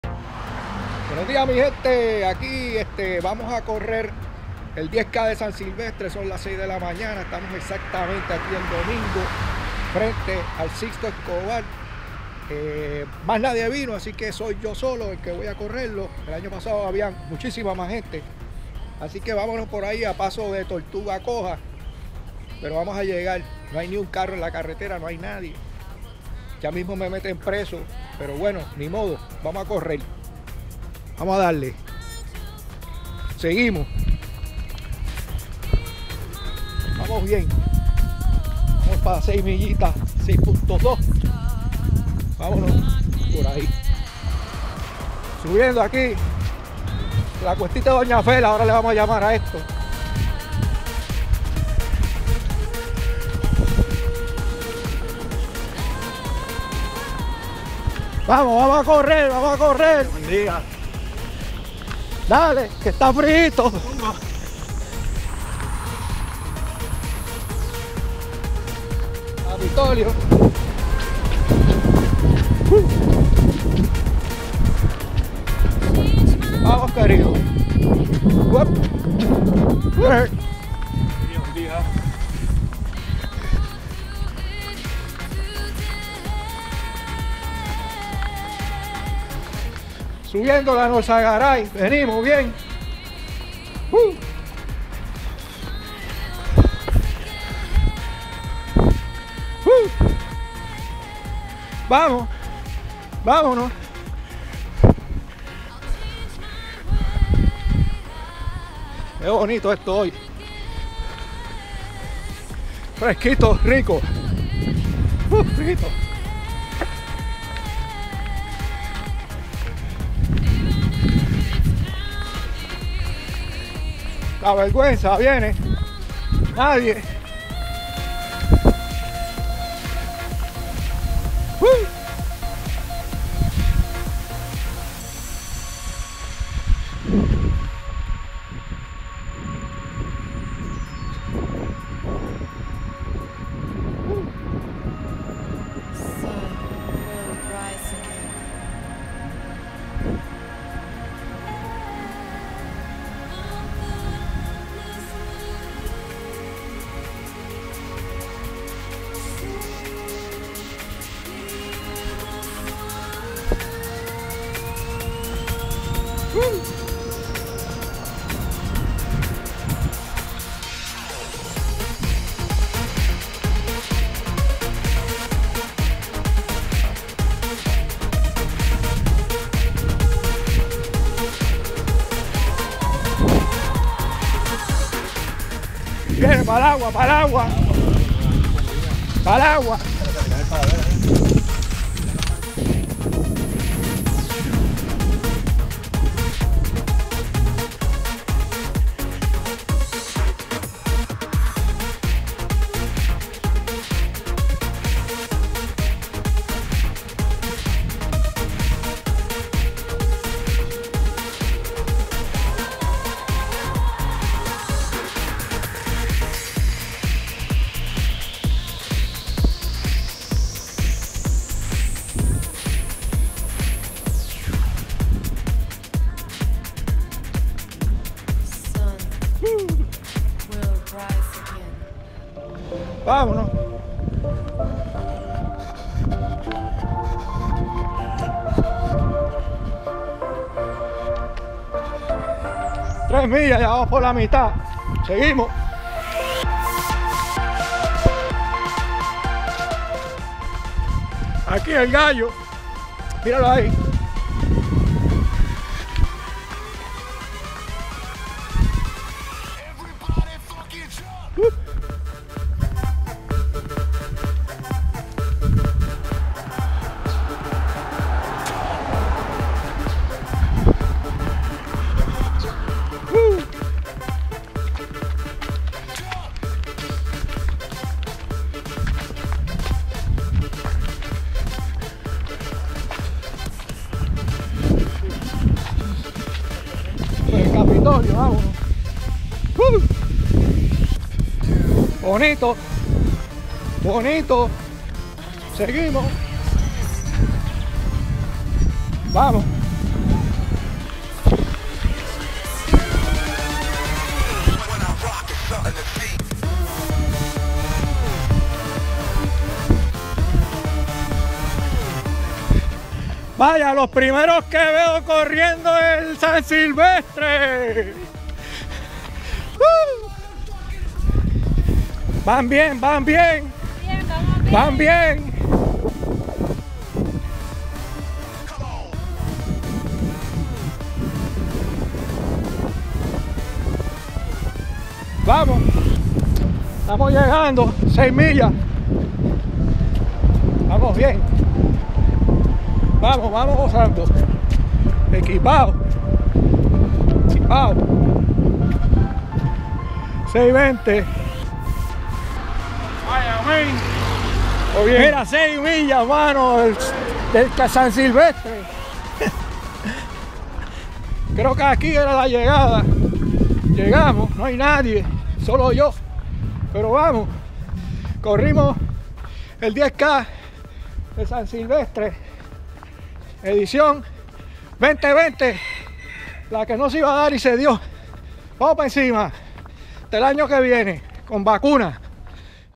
10K San Silvestre 2020 San Juan, Puerto Rico - Mi carrera!!
Corriendo con la GoPro la edición virtual del San Silvestre